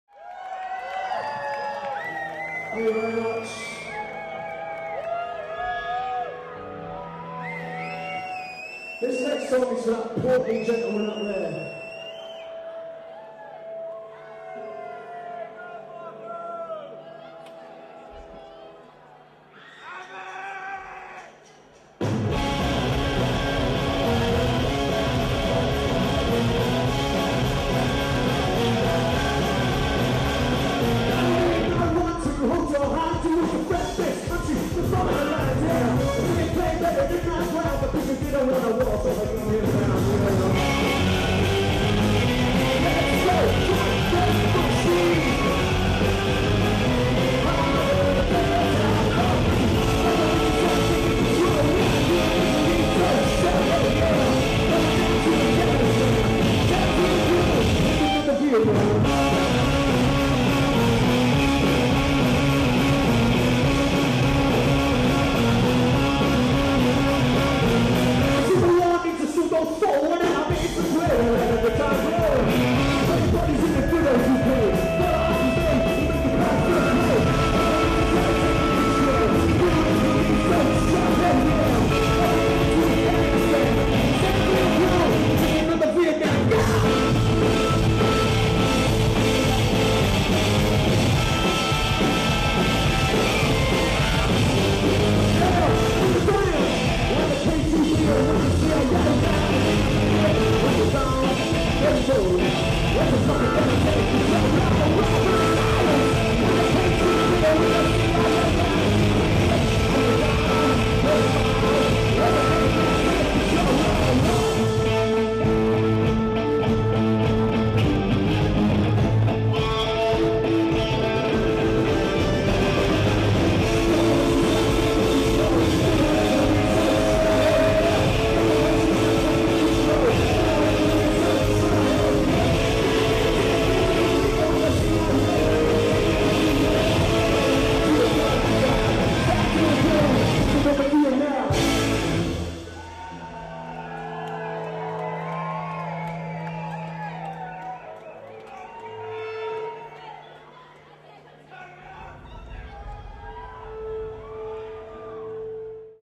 White Man Kamikaze - Club NME - KOKO 1st May 2009
WhiteMan Kamikaze playing at KoKo ,Camden, London